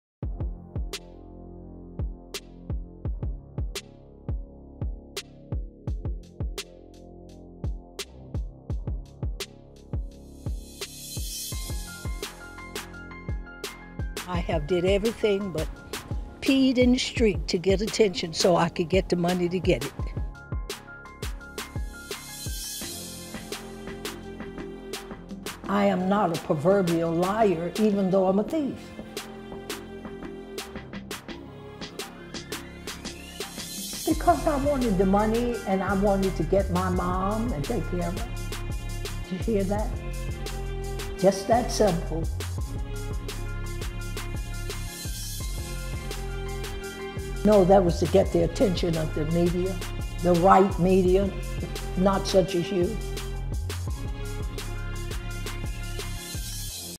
86-Year-Old International Jewel Thief Doris Payne A Free Woman After Judge Dismissed Theft Charges
Doris Payne tells says she stole jewels to help her mother.